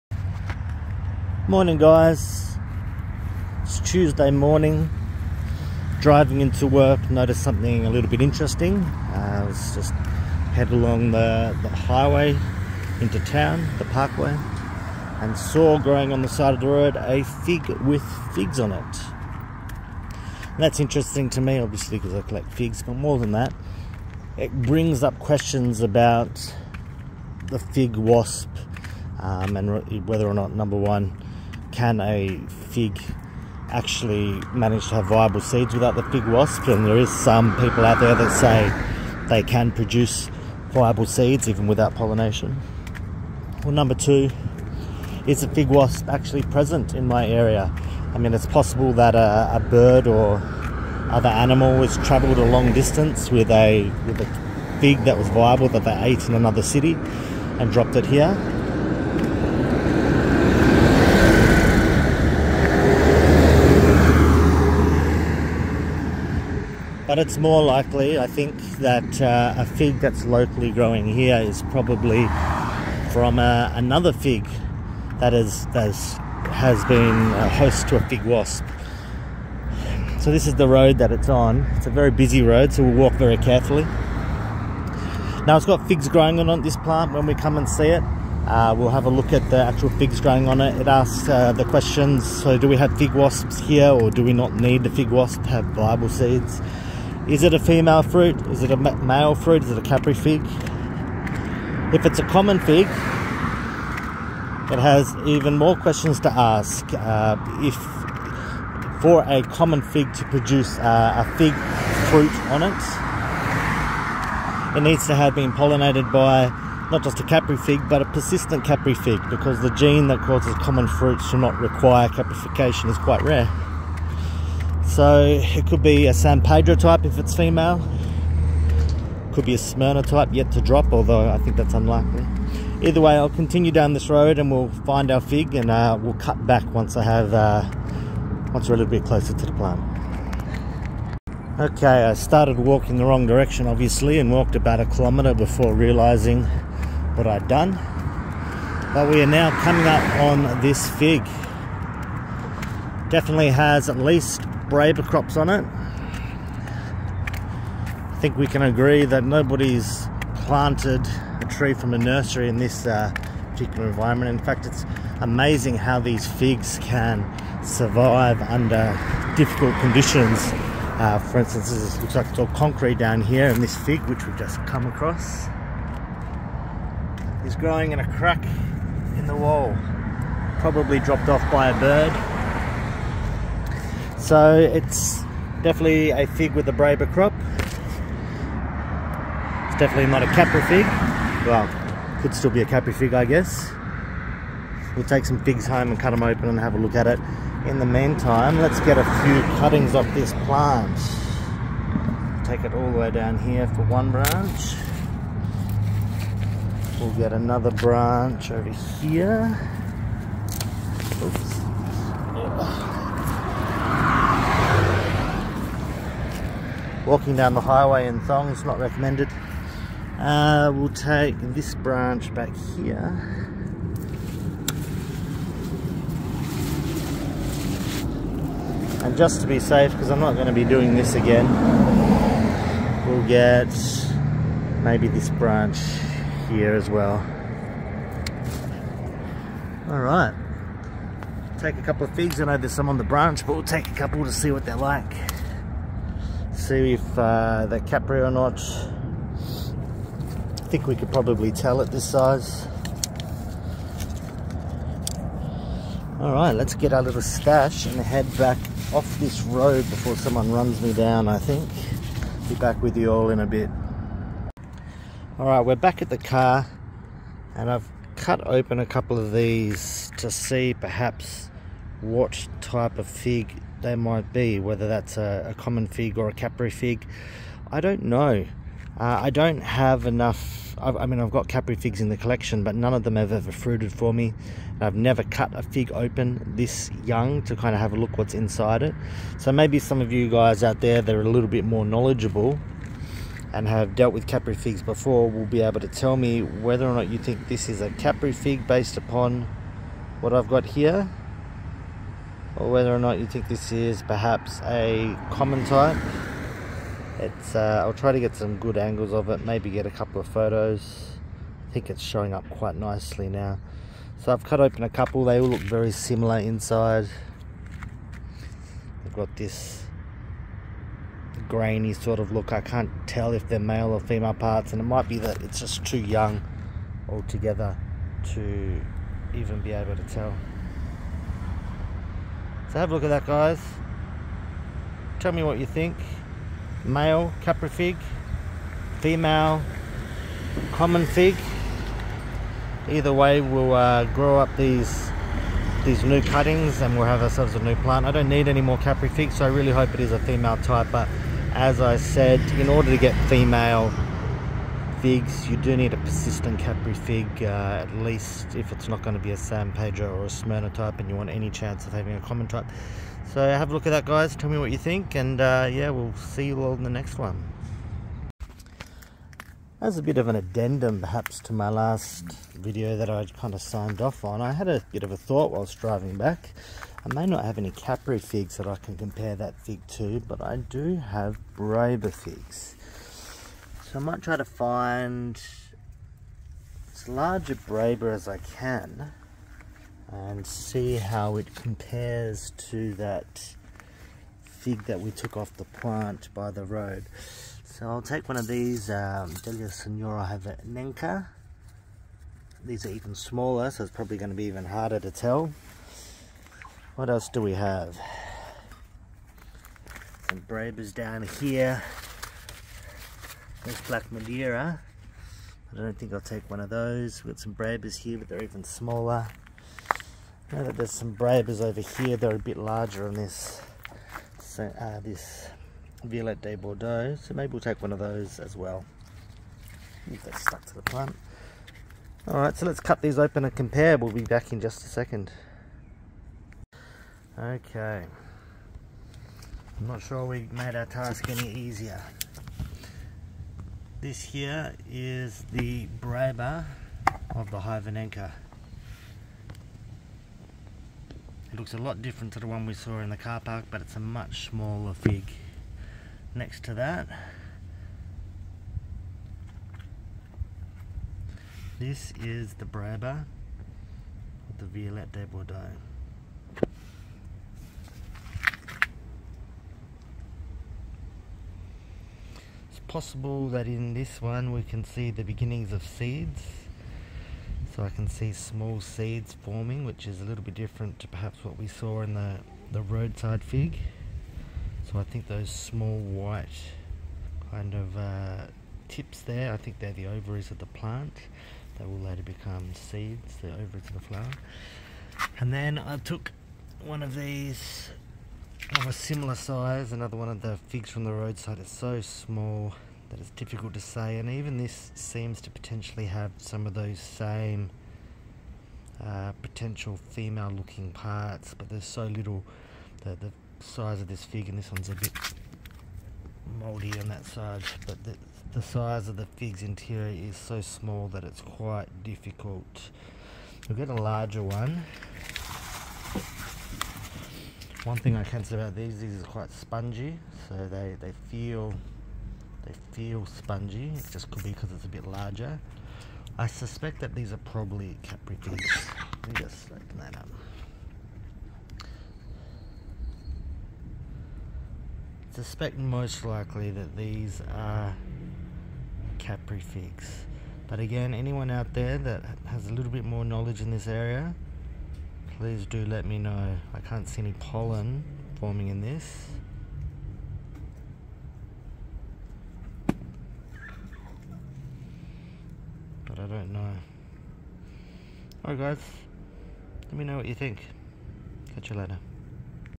Mystery UNK Roadside Fig. Caprifig or Common?
On the way into the office drove past a fruiting fig. THis fig has large Breba or Caprifigs on it. Took some cuttings and would like to know everyone's thoughts on whether or not it is a capri or other type of fig.